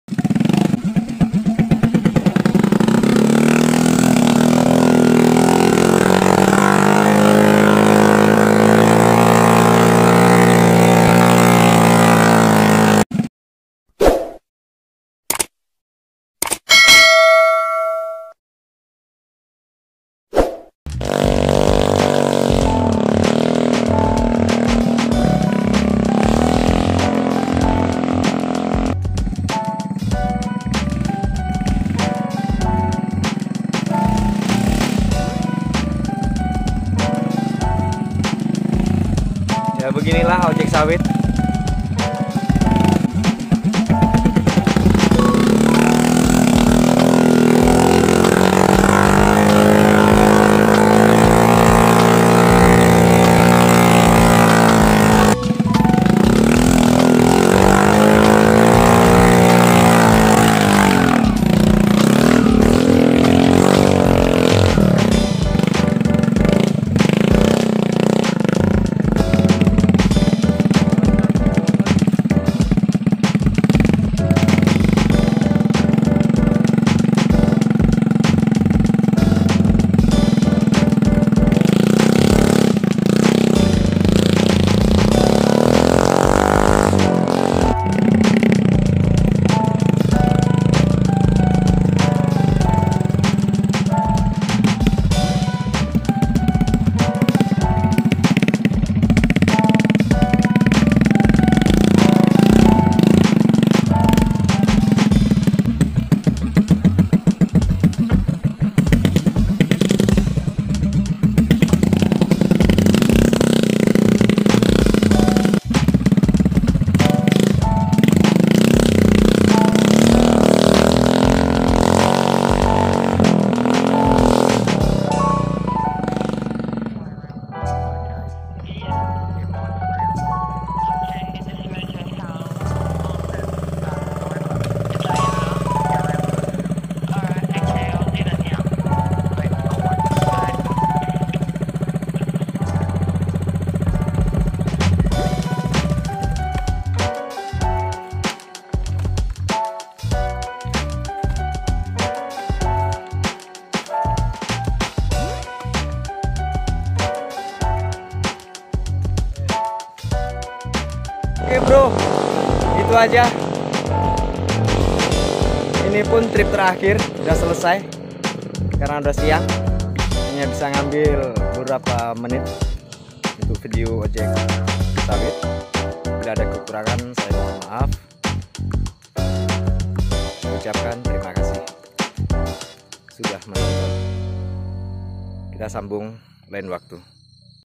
THE MASTER of OJEK SAWIT ‼️(Baskoro Si Kakek Merah)
Lansir Sawit dengan menggunakan sepeda motor.